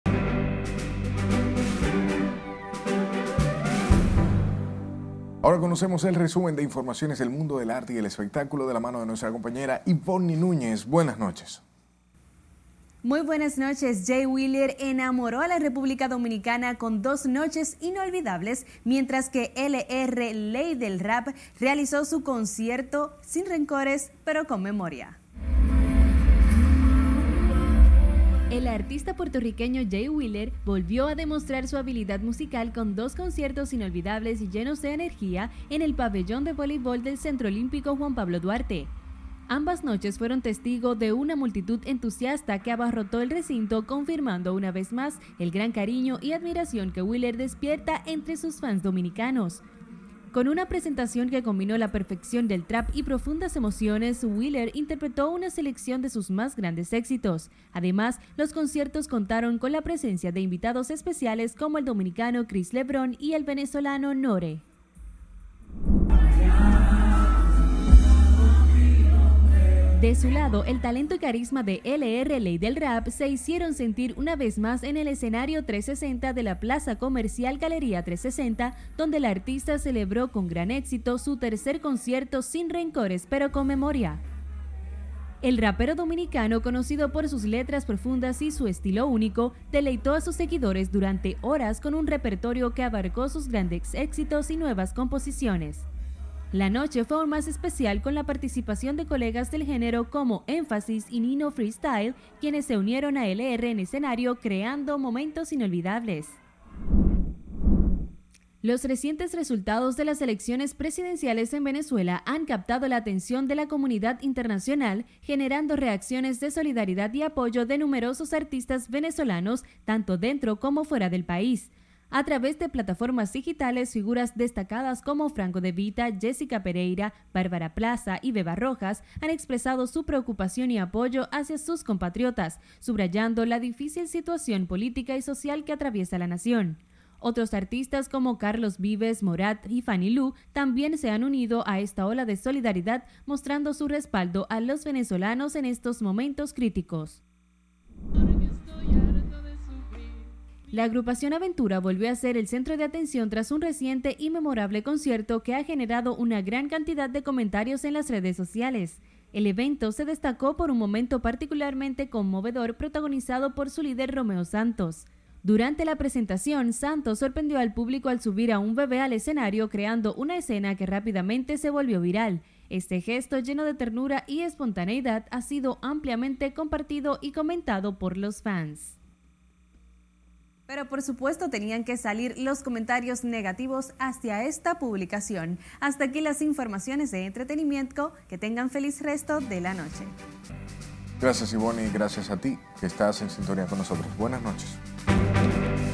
Jay Wheeler enamora a RD con dos conciertos inolvidables
El artista puertorriqueño Jay Wheeler volvió demostrar su habilidad musical con dos conciertos inolvidables y llenos de energía en el Pabellón de Voleibol del Centro Olímpico Juan Pablo Duarte. Ambas noches fueron testigo de una multitud entusiasta que abarrotó el recinto, confirmando una vez más el gran cariño y admiración que Wheeler despierta entre sus fans dominicanos.